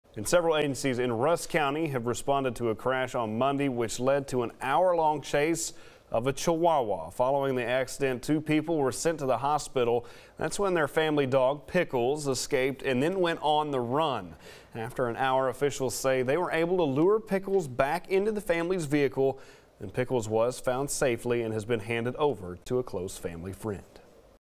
Family chihuahua found safe after Gun Barrel City crash
The Gun Barrel City Fire Department and other agencies responded to a crash that led to an hour-long chase of a chihuahua on Monday.